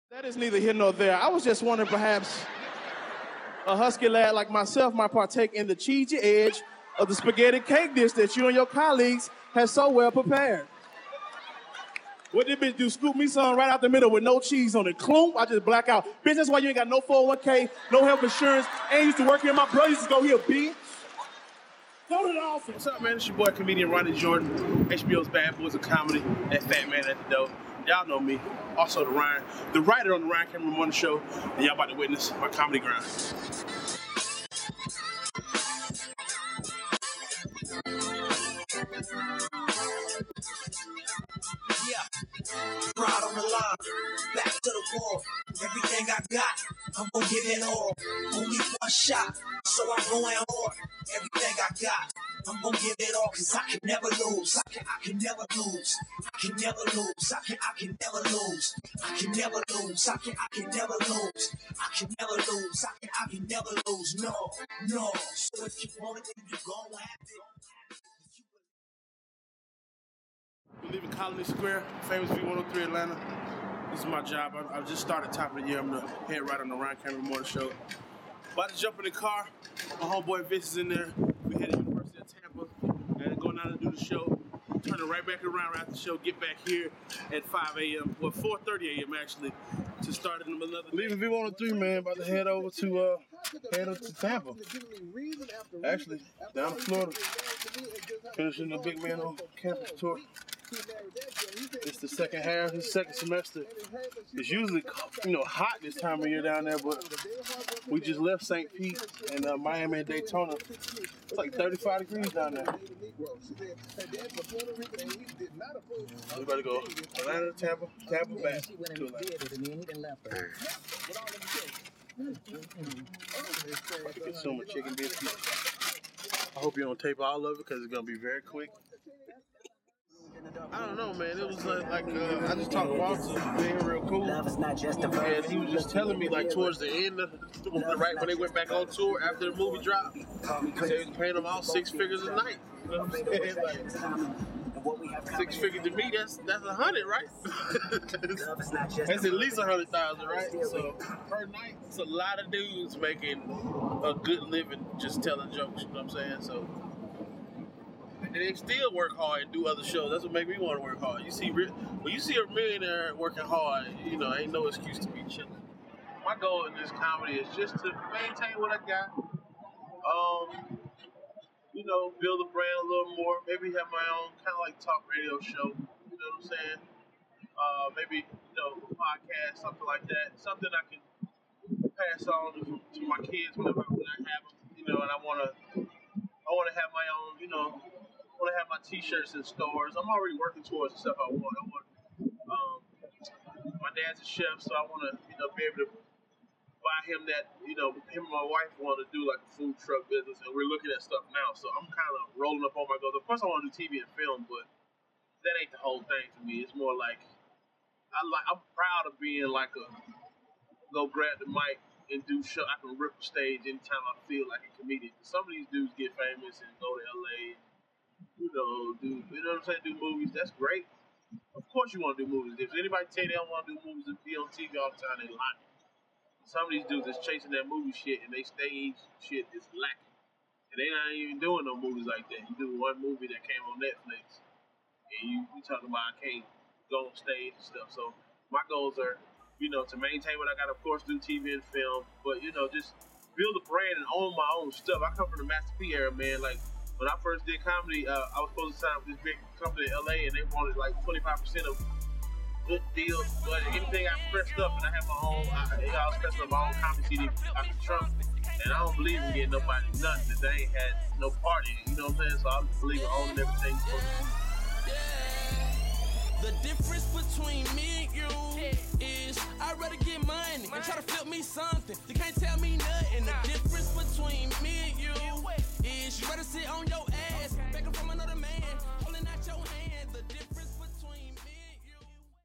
Comedy Grind Episode 3: Comedian Ronnie Jordan Part 1
- We join Ronnie Jordan as he travels from Atlanta to Tampa and back to Atlanta in 24Hrs for his 2013 Big Man On Campus Tour.